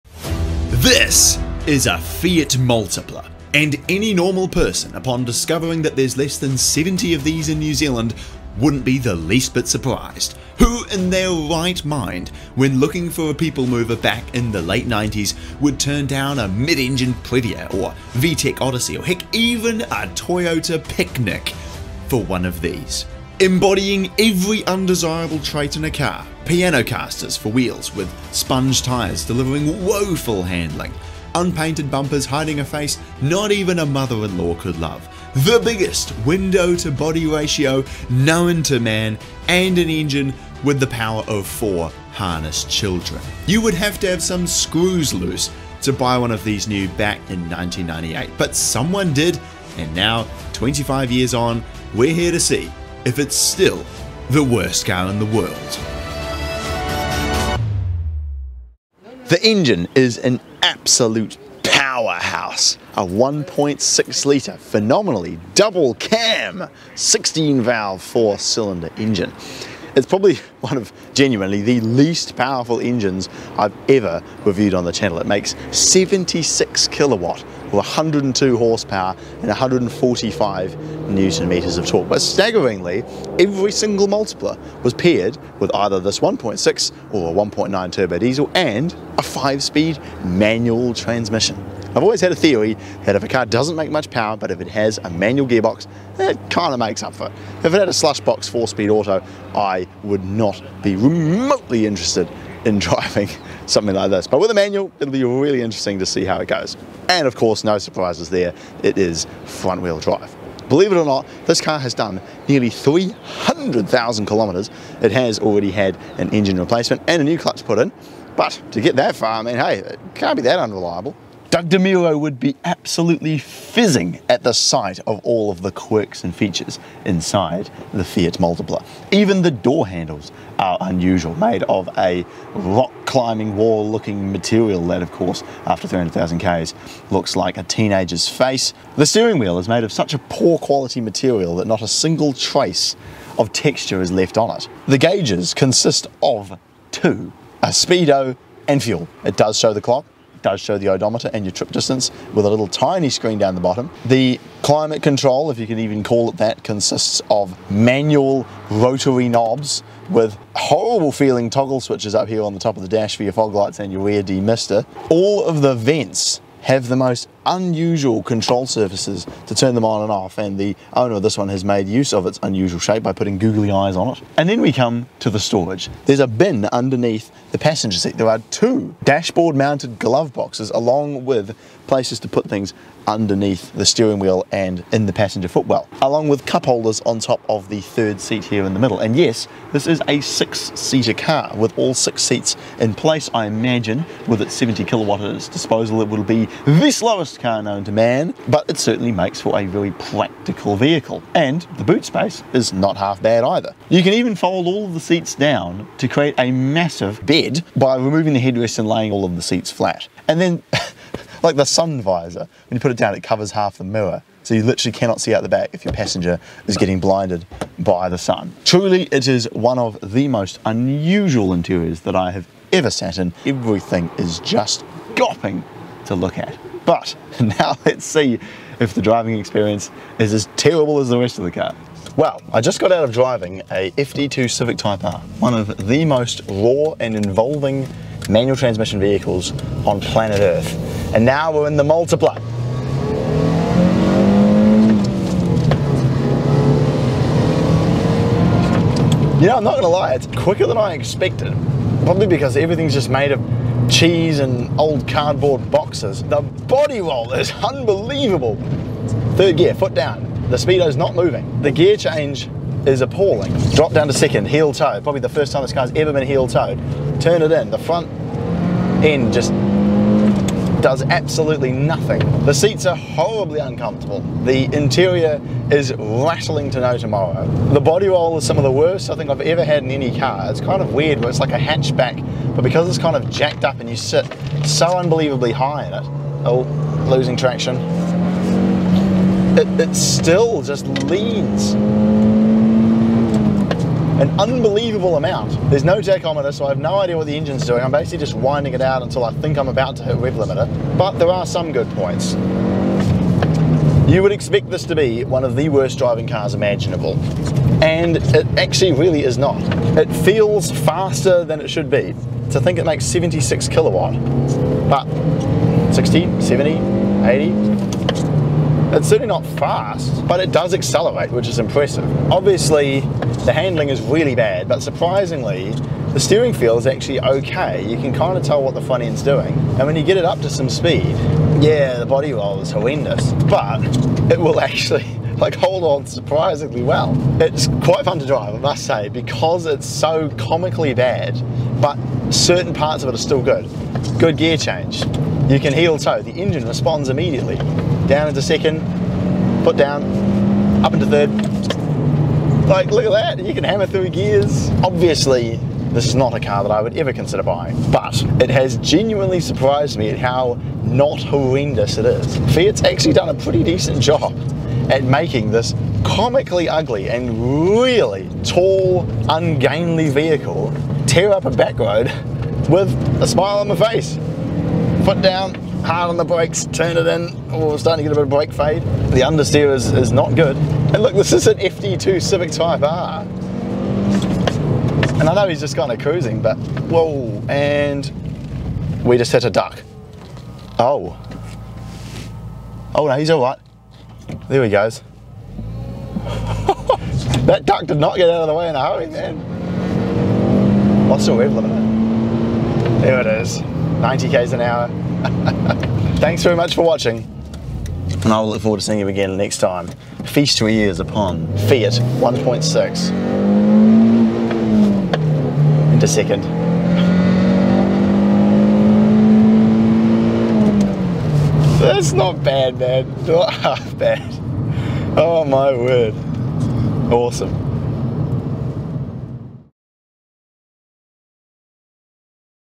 Is the Fiat Multipla the WORST Car in the World?
Today I review a Fiat Multipla to see if the Fiat Multipla really is the worst car ever made.

Chapters:
0:00 Intro
0:59 Drivetrain
2:01 Interior
3:52 Driving Experience